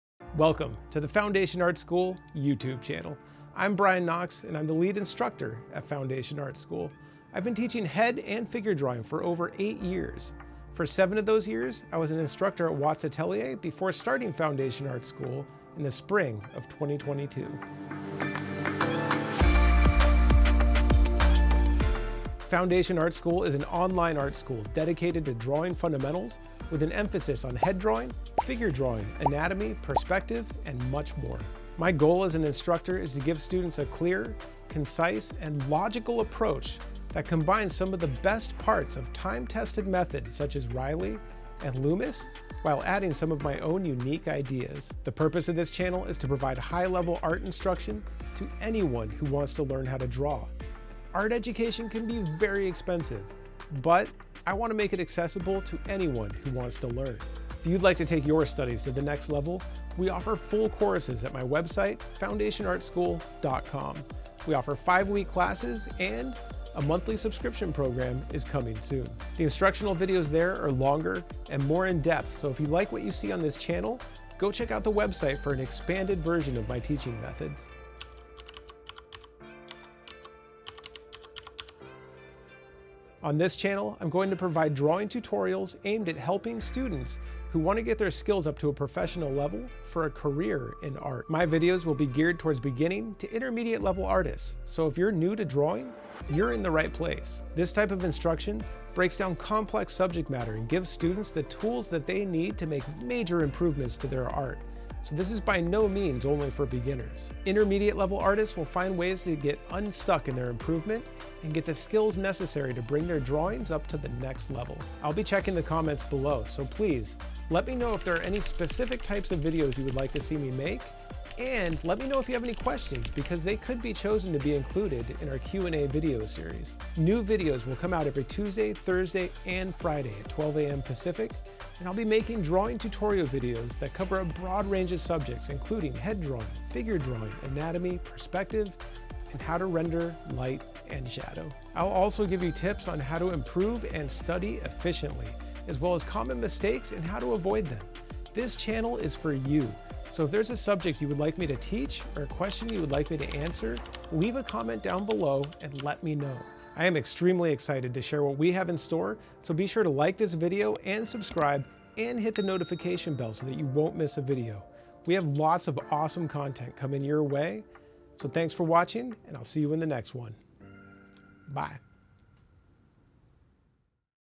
Welcome to the Foundation Art School YouTube Channel!
Welcome to the Foundation Art School YouTube channel! My partner, Olivia, and I are excited to launch this channel as it allows me to share my knowledge with a wider audience and help more artists to succeed.

Be sure to leave a comment to let us know what videos you want to see! This channel is for you, so let us know what you want to see and we will create videos based off of your requests.